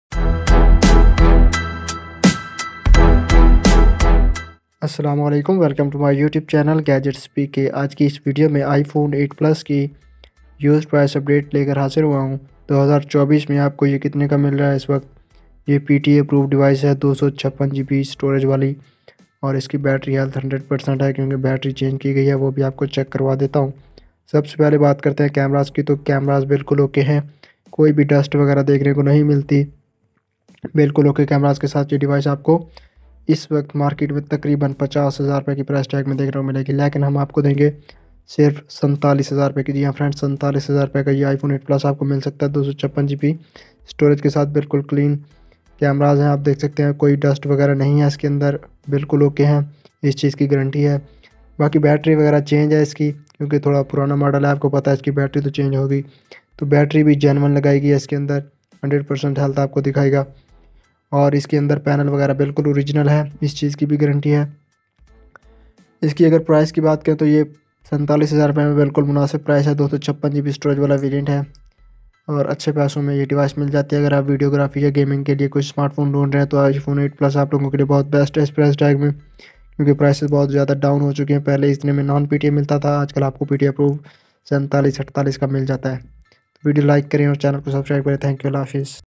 Iphone 8 Plus PTA Approved ✅ 256GB used price perfect device for camera and gaming lovers
"Get the latest update on the prices of used iPhone 8 Plus 256GB in Pakistan! In this video, we'll reveal the current market price of PTA approved, 256GB iPhone 8 Plus in Pakistan as of 2024. Whether you're looking to buy or sell, this video will give you the insider information you need to make an informed decision. Watch till the end to know the exact price and make a smart move in the market!